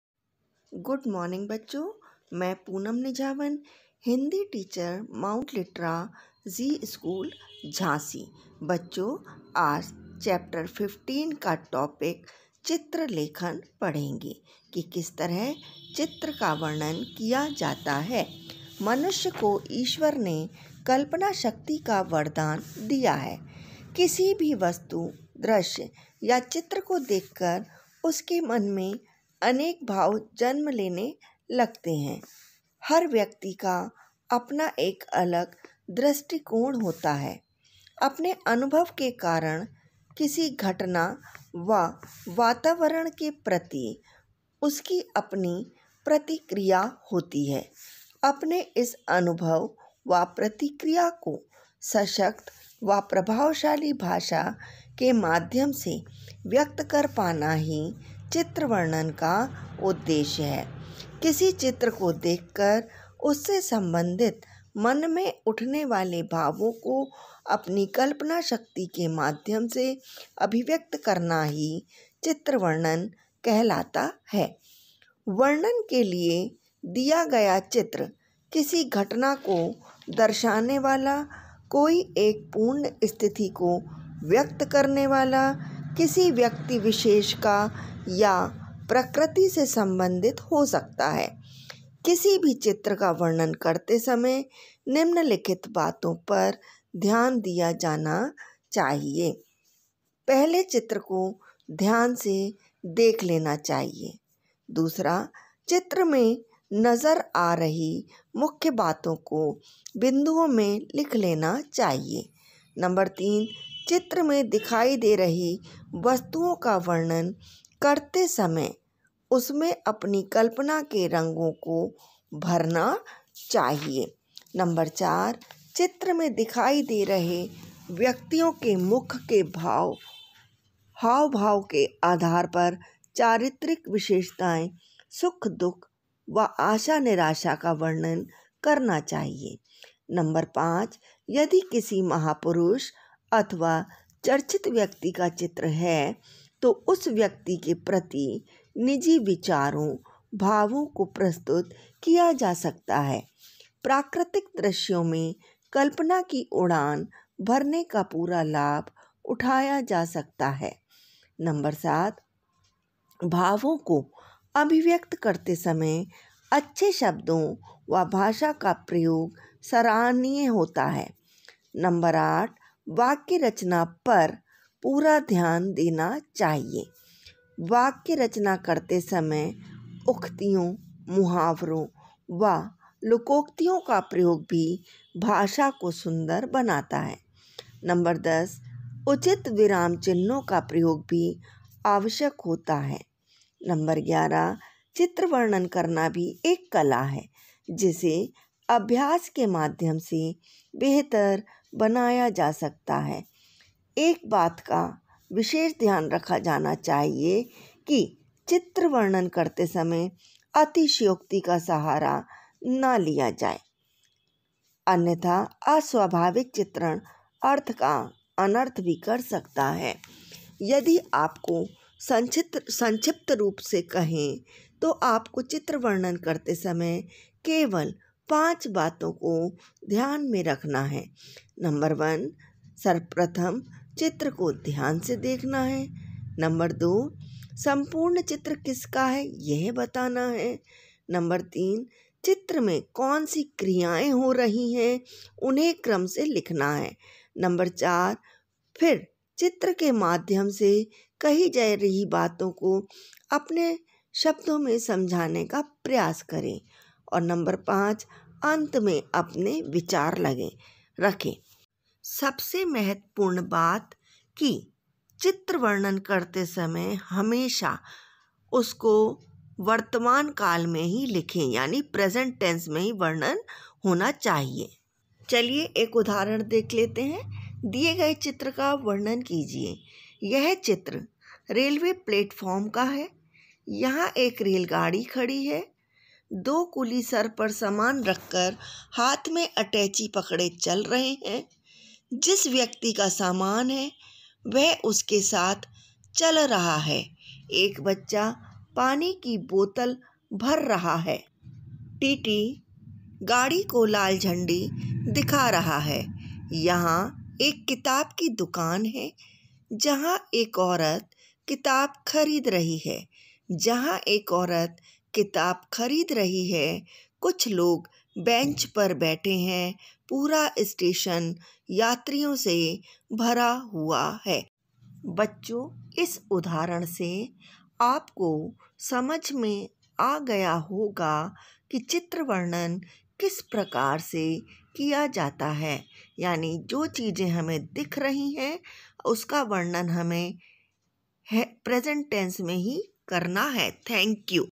MLZS JHANSI CLASS 7 Hindi Grammar Chapter 15 Rachnatmak Abhivyakti Chitra Lekhan
MLZS JHANSI CLASS 7 Hindi Grammar Chapter 15 Rachnatmak Abhivyakti Chitra Lekhan By Poonam Mam